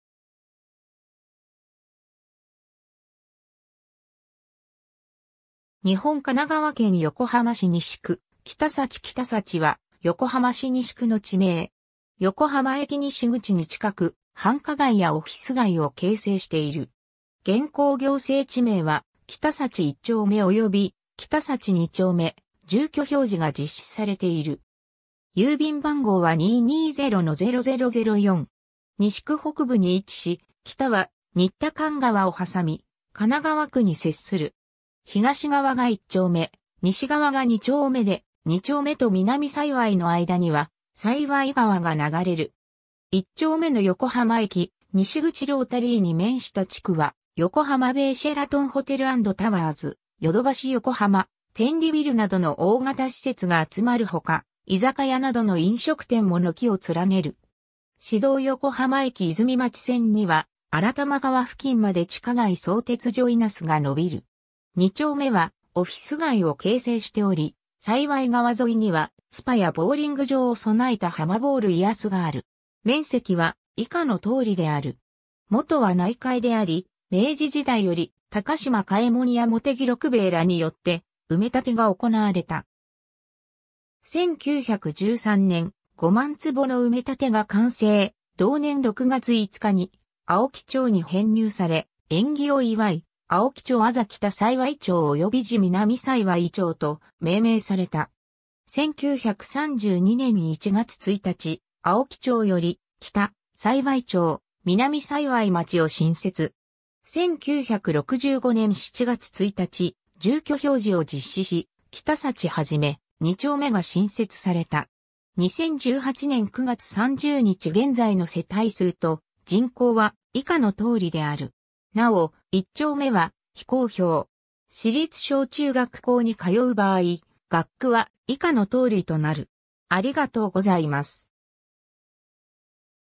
北幸
日本 」 神奈川県 」 横浜市 」 西区 」 北幸 北幸（きたさいわい）は、横浜市西区の地名。
横浜駅西口に近く、繁華街やオフィス街を形成している。
現行行政地名は北幸一丁目及び北幸二丁目、住居表示が実施されている。
 西区北部に位置し、北は新田間川を挟み神奈川区に接する。
東側が一丁目・西側が二丁目で、二丁目と南幸の間には幸川が流れる。
 一丁目の横浜駅西口ロータリーに面した地区は横浜ベイシェラトン ホテル&タワーズ、ヨドバシ横浜、天理ビルなどの大型施設が集まるほか、居酒屋などの飲食店も軒を連ねる。
市道横浜駅泉町線には新田間川付近まで地下街相鉄ジョイナスが伸びる。
二丁目はオフィス街を形成しており、幸川沿いにはスパやボウリング場を備えたハマボール イアスがある。
 面積は以下の通りである。
 もとは内海であり、明治時代より高島嘉右衛門や茂木六兵衛らによって埋立てが行われた。
1913年（大正2年）、5万坪の埋立が完成、同年6月5日に青木町に編入され、縁起を祝い、青木町字北幸町および字南幸町と命名された。
1932年（昭和7年）1月1日、青木町より北幸町・南幸町を新設。
1965年7月1日、住居表示を実施し、北幸一・二丁目が新設された。
 2018年（平成30年）9月30日現在の世帯数と人口は以下の通りである。
なお、一丁目は非公表。